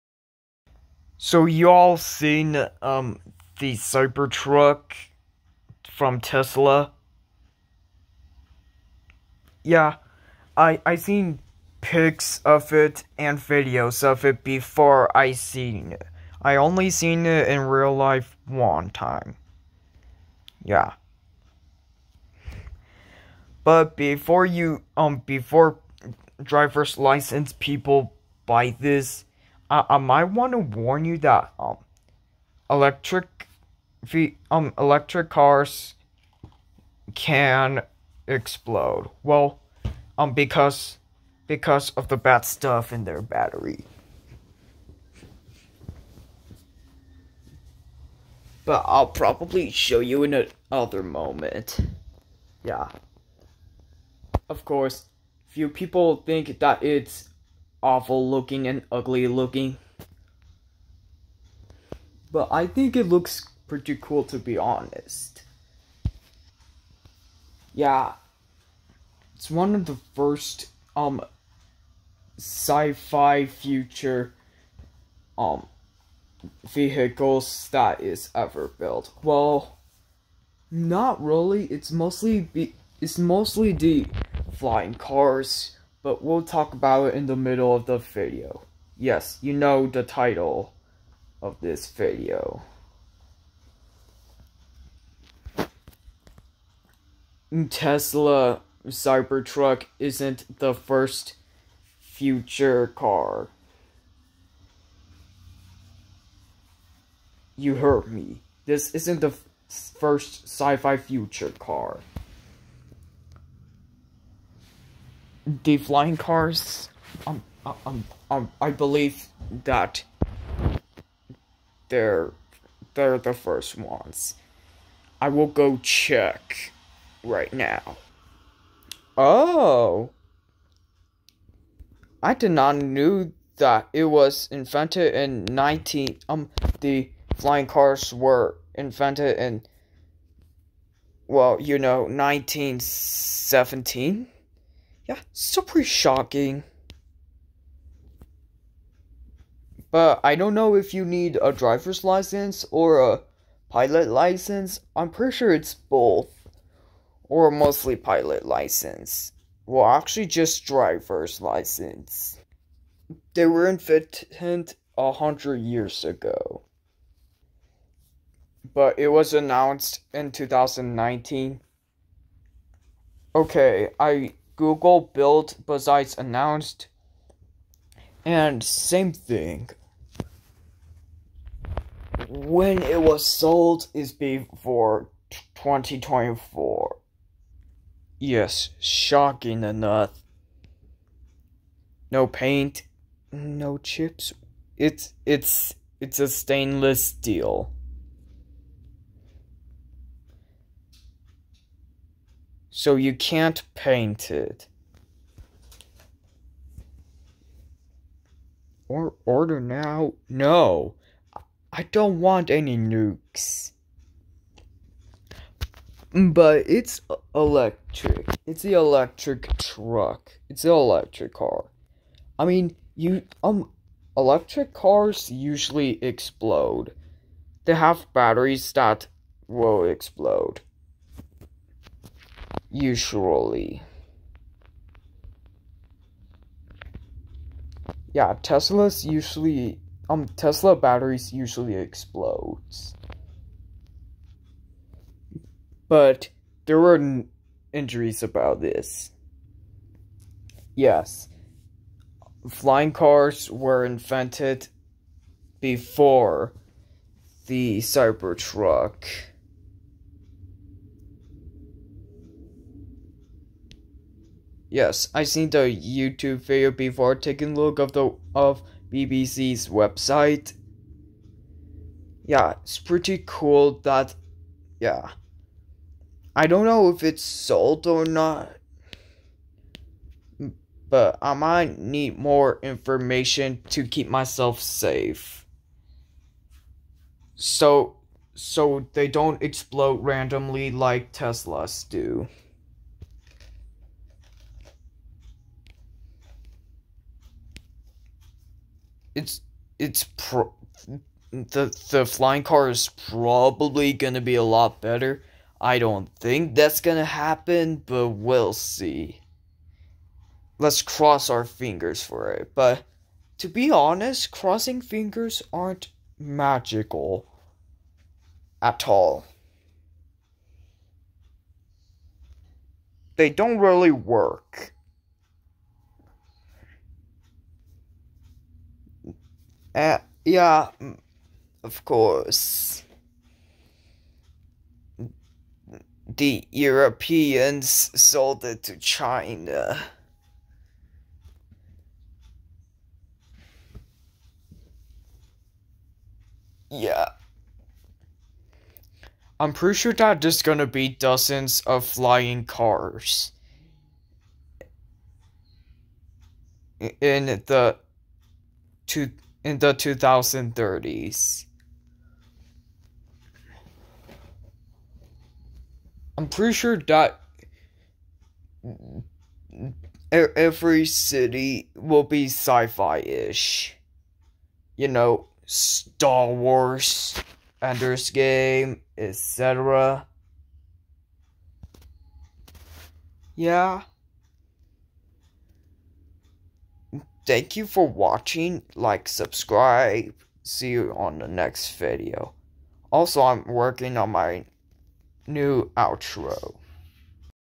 The Tesla Cybertruck Isn't The First Sci-fi Future Car
Flying Cars Is Probably Gonna Be Alot Better Than The Cybertruck, Hopefully.

IDK